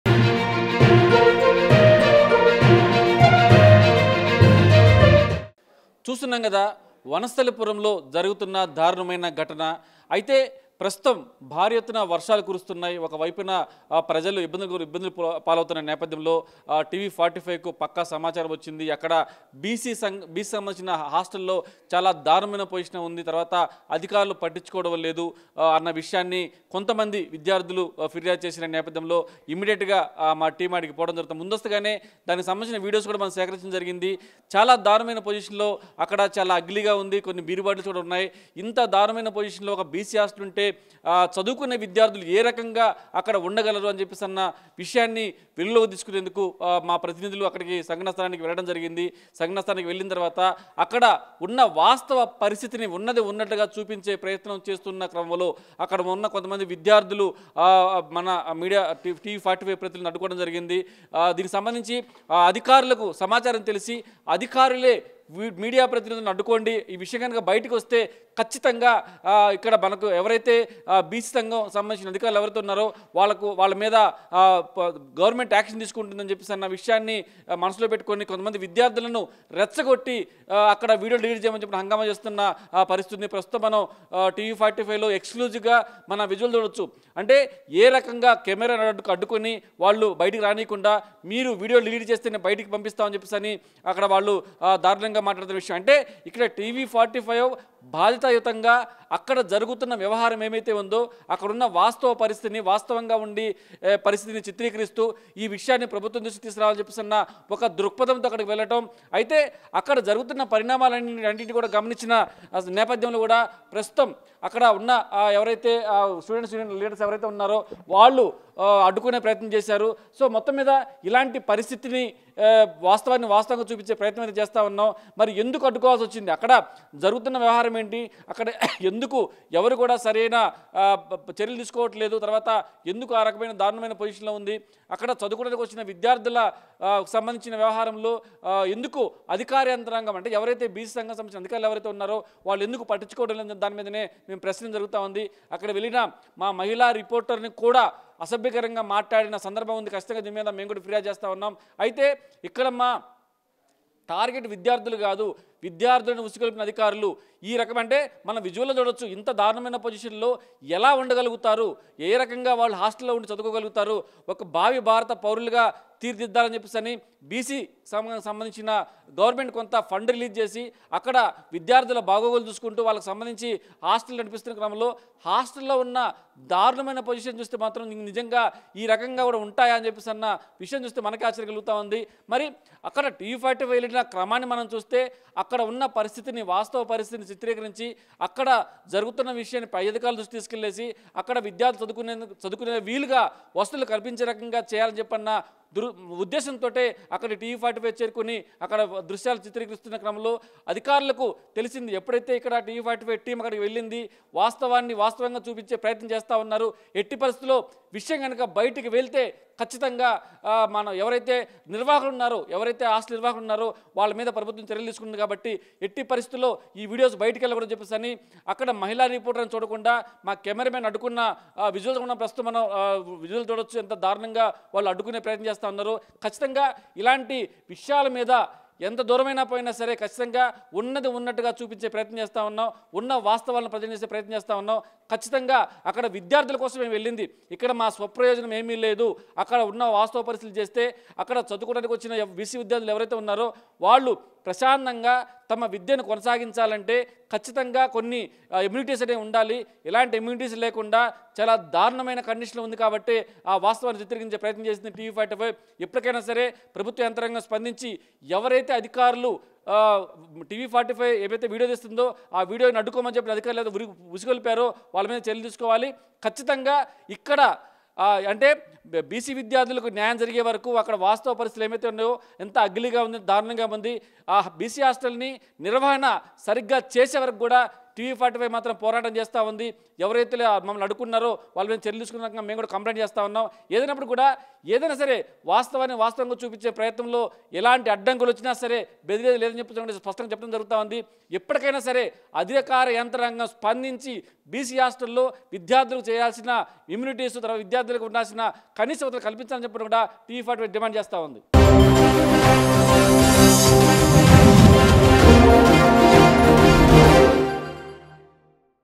Vanasthalipuram BC Hostels Latest News | Vanasthalipuram BC Boys Hostel Hyderabad
వనస్థలిపురం BC హాస్టల్ లో  దారుణం కవర్ చేస్తున్న TV 45 ప్రతినిధి పై దాడికి యత్నం